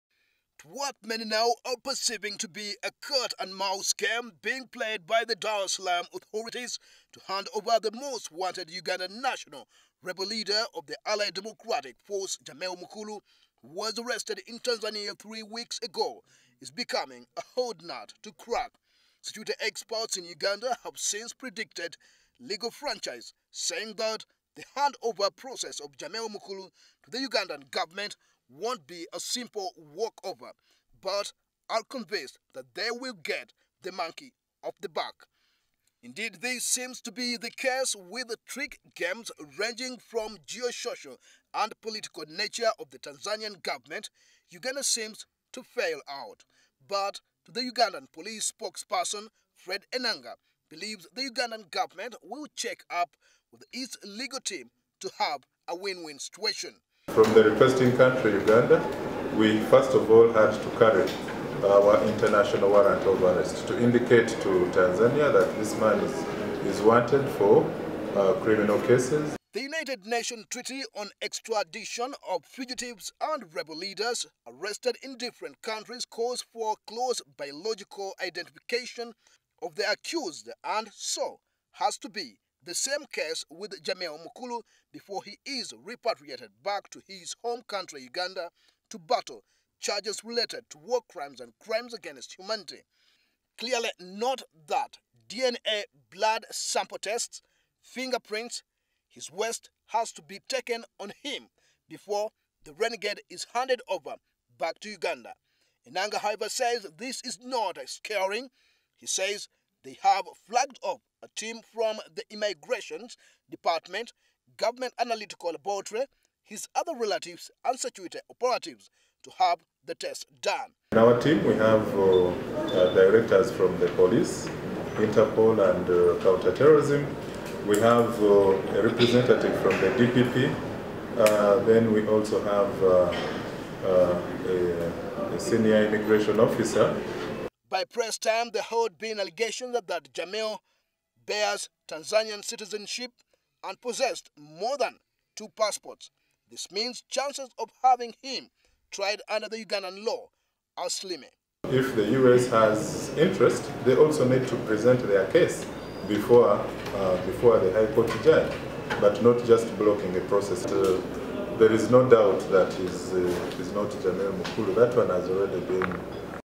The Tanzania government has called for documentation ascertaining Jamil Mukulu is truly Ugandan
The Tanzania government has called for clear documentation ascertaining that renegade rebel leader of the allied democratic forces is truly Ugandan before they hand him over to Uganda.

Tanzania’s demand for Mukulu’s DNA taste and other documents followed America’s intervention into the extraditing process demanding for the two countries Tanzania and Uganda to follow extradition treaty of the United Nations Security Council which calls for maximum identification of the suspect in custody before handing him over to his respective country.

With this latest demands that seem to attract international attention, the treaty calls for a thorough DNA sample test and finger print tastes which the Uganda government says is ready to provide.